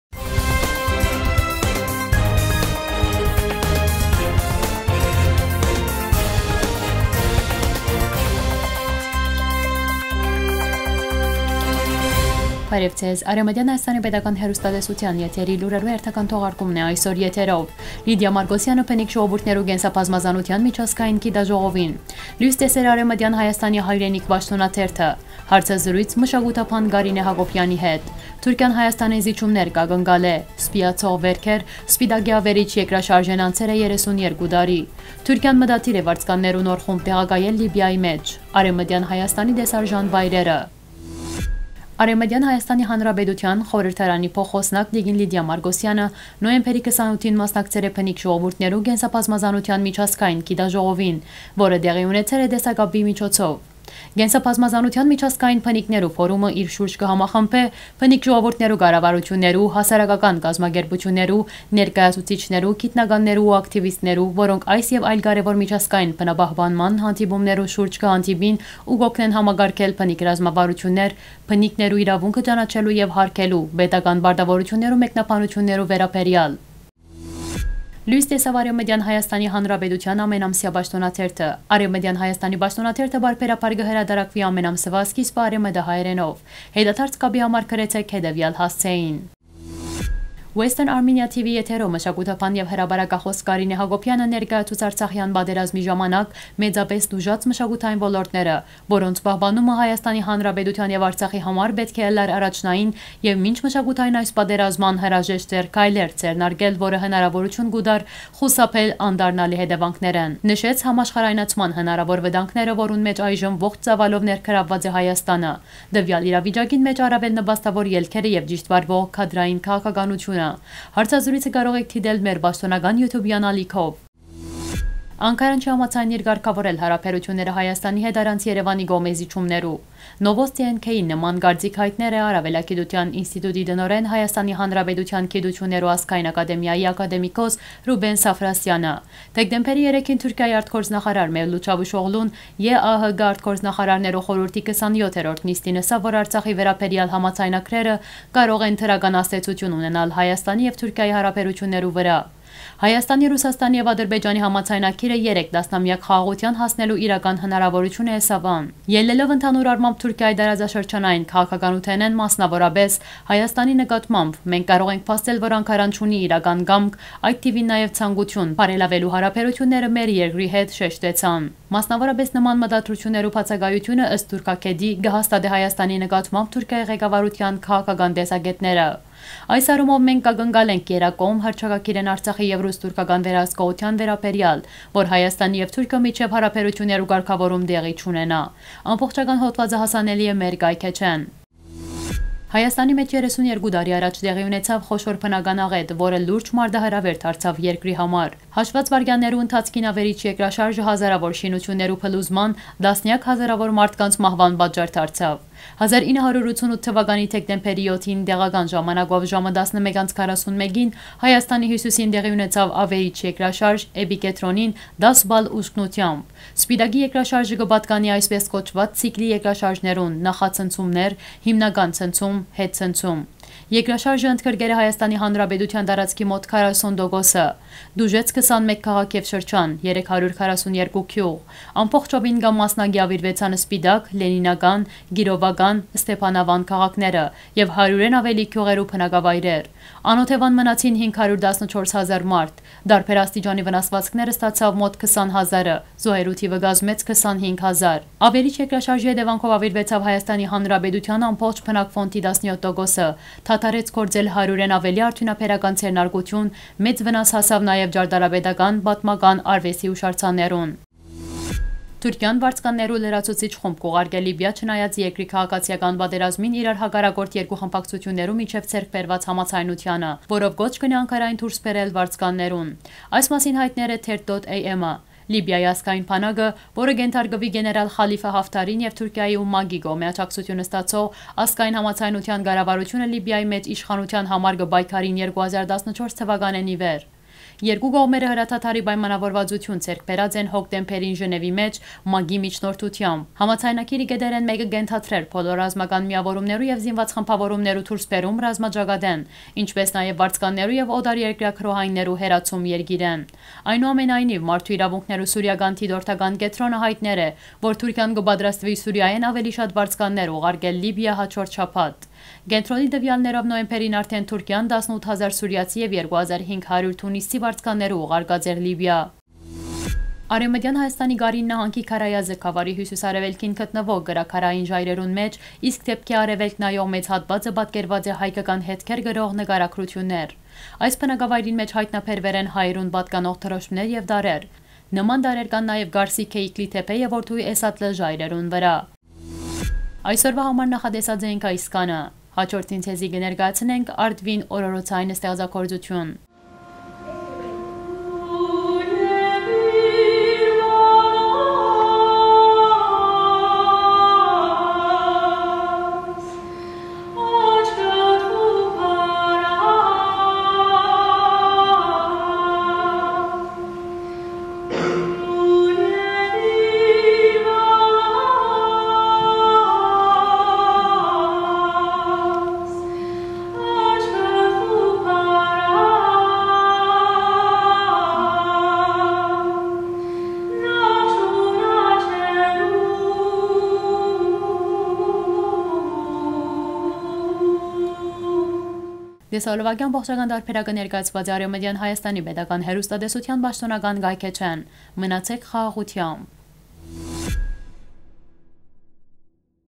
Թուրքիան Հայաստանէն զիջումներ կակնկալէ.Լուրեր 07 12 2020
1․Լիտիա Մարկոսեանը Բնիկ Ժողովուրդներու  Կենսաբազմազանութեան Միջազգային Գիտաժողովին
2.Լոյս տեսեր է Արեւմտեան Հայաստանի Հայրենիք պաշտօնաթերթը
3.Հարցազրոյց մշակութաբան Կարինէ Հակոբեանի հետ
4․Թուրքիան Հայաստանէն զիջումներ  կակնկալէ
5.Սպիացող վերքեր. Սպիտակի աւերիչ երկրաշարժէն անցեր է 32 տարի
6.Թուրքիան մտադիր է վարձկաններու նոր խումբ տեղակայել Լիբիայի մէջ
7.Արեւմտեան Հայաստանի տեսարժան վայրերը․Կարինի Ջուննի քարանձաւը

WESTERN ARMENIA FOUNDATION
CNA
IBAN : FR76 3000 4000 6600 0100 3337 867 - BIC : BNPAFRPPMAS